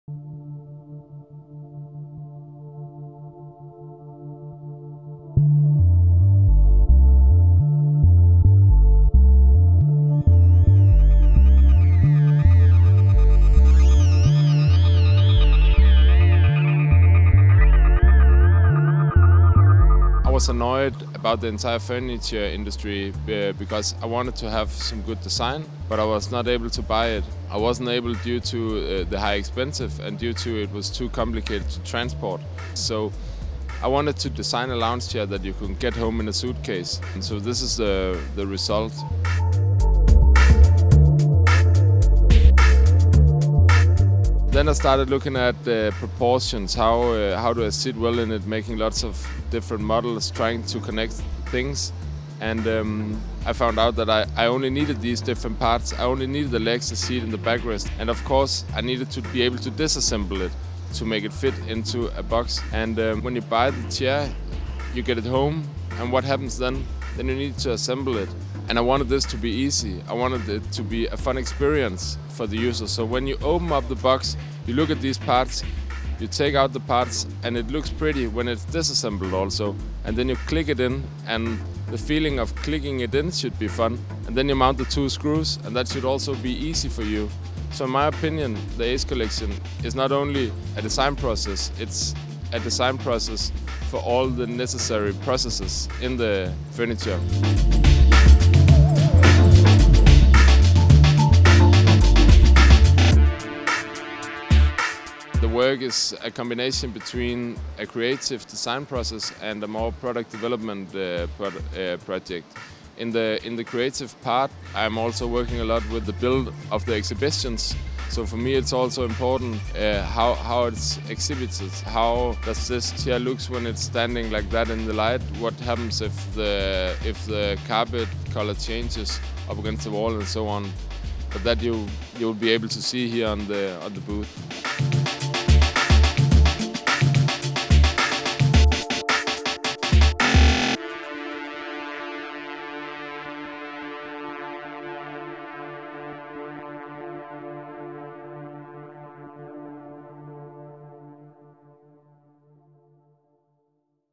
Salone del Mobile.Milano 2016 | NORMANN COPENHAGEN - Hans Hornemann
Designer Hans Hornemann talks about Ace the new collection by Normann Copenhagen. Interview by Archiproducts Staff in Milano during the Milano Design Week 2016.